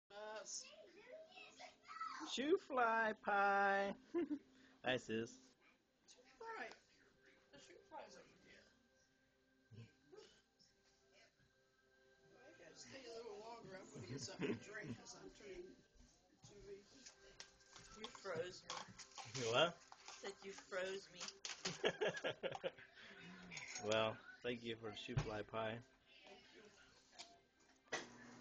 Shoo-Fly Pie from my sister
My sister made some Shoo-Fly Pie.